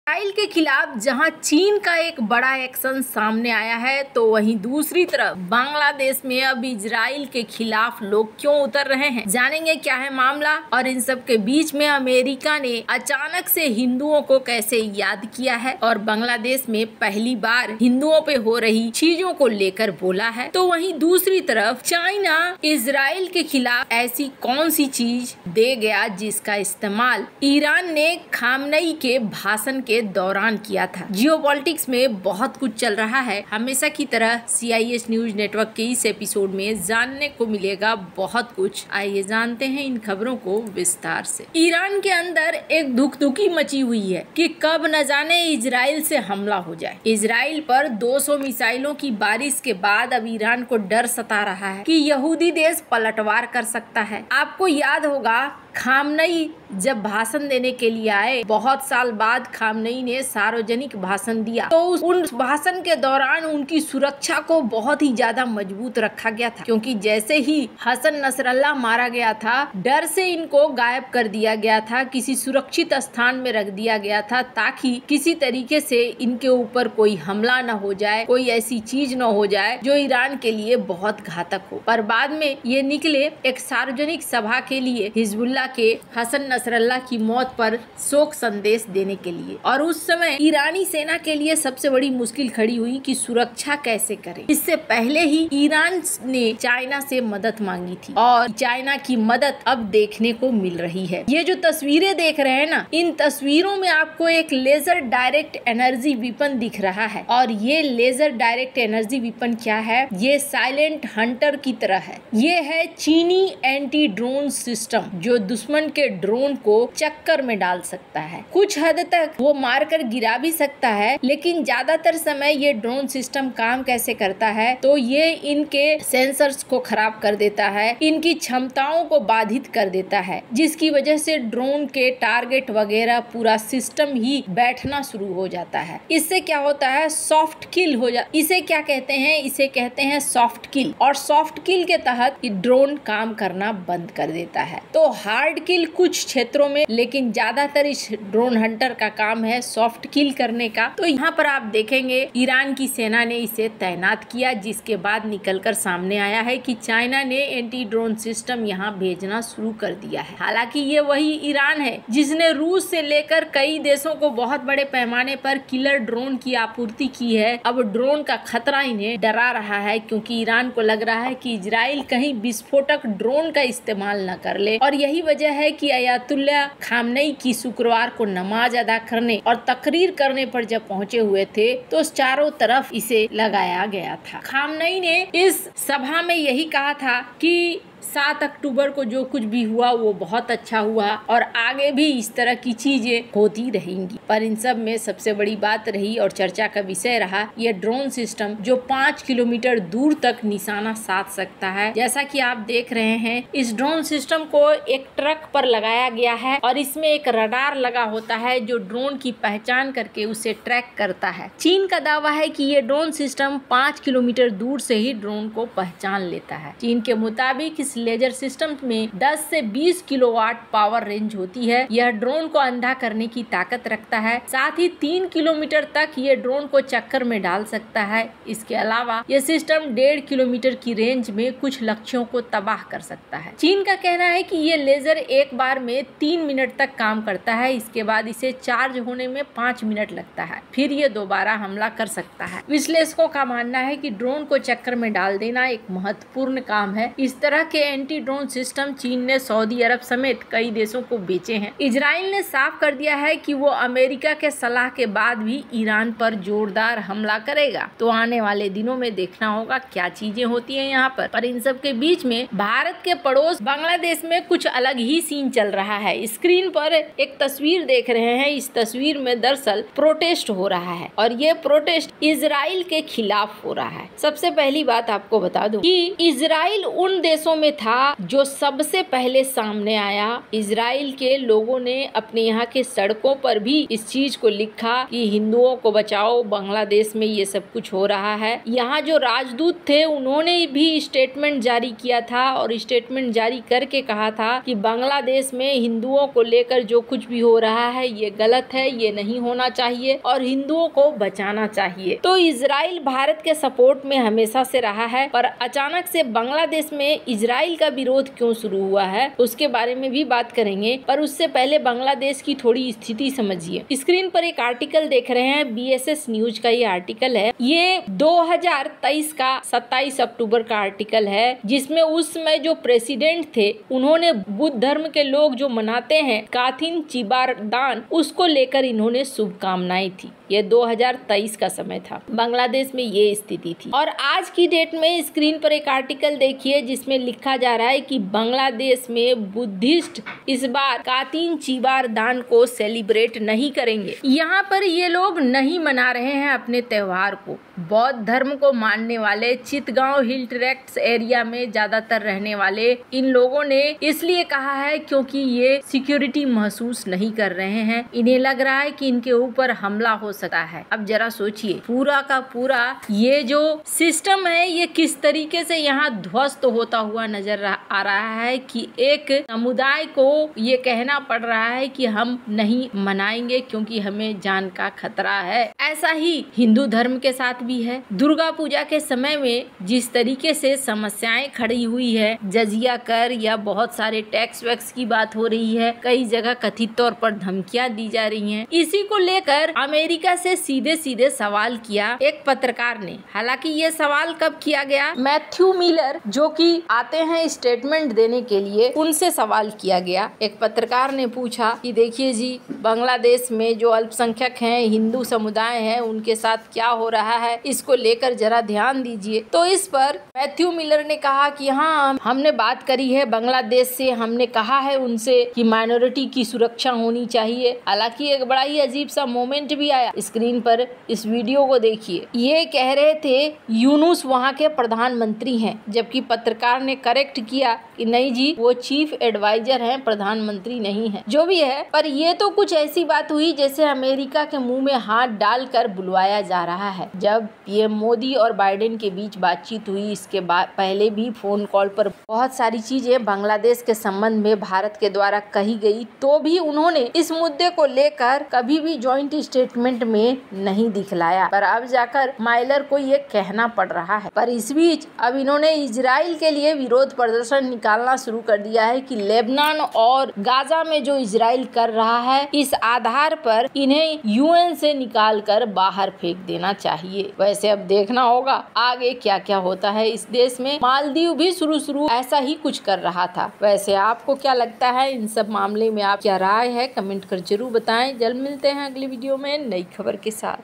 बंगलादेश संभल जाओ : US | इजराइल के खिलाफ उतरा बंगलादेश | चीन ने भेजा ईरान खास हथियार
In this video we have analyzed two updates :
1. Iran Deploys Now Chinese Laser Weapon System to Protect High-Value Targets Against Aerial Threats.
2. US urges Bangladesh to protect rights of Hindus as Durga Puja celebrations begin